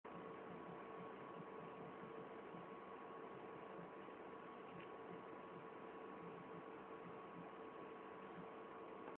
Flame hand
My first attempt with Chroma key and explosion from detonation films